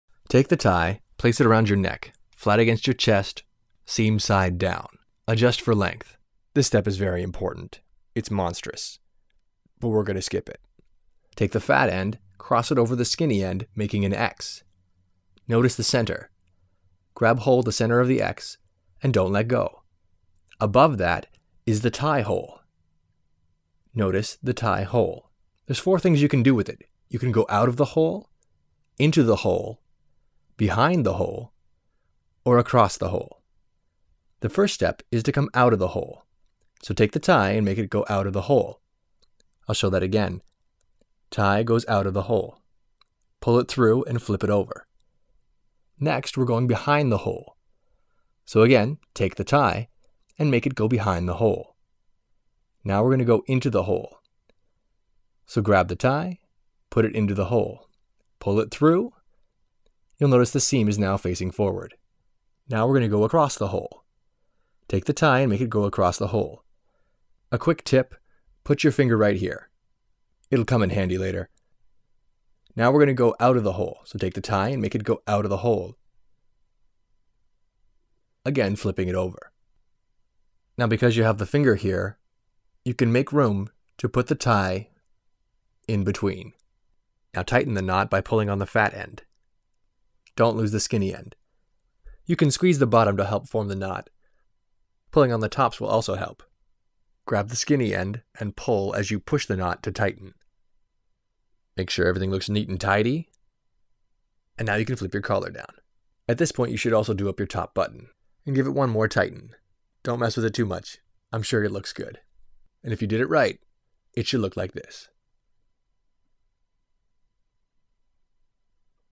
How to Tie a Windsor Knot - 2 minute version
This is a demonstration of how to tie a Full Windsor Knot using the "tie hole" method.  For the Full Windsor the pattern is:

Out, Behind, In, Across, Out.

This should allow you to be able to remember how to tie a tie without having to watch the video every time.